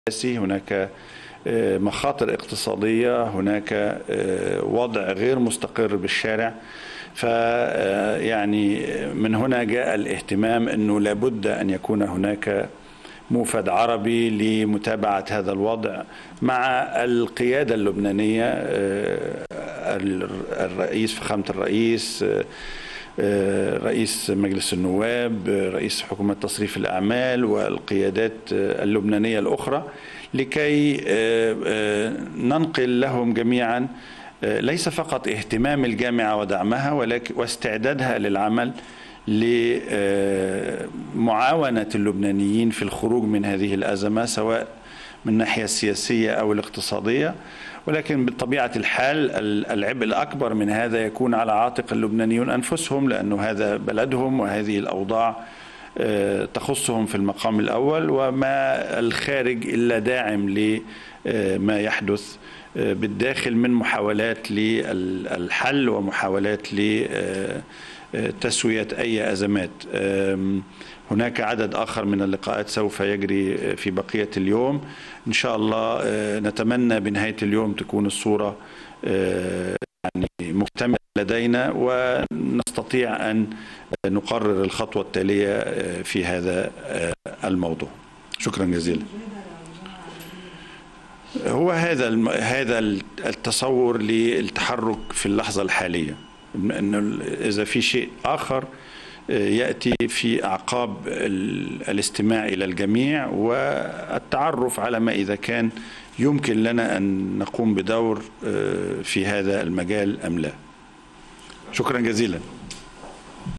موفد من الجامعة العربية يزور بعبدا: مستعدون للعمل لمعاونة لبنان للخروج من الأزمة
استقبل رئيس الجمهورية العماد ميشال عون في قصر بعبدا الأمين العام المساعد لجامعة الدول العربية السفير حسام زكي، وعرض معه الأوضاع العامة في البلاد، ونقل اليه رسالة دعم من الجامعة العربية واستعدادها للمساعدة في حل الأزمة اللبنانية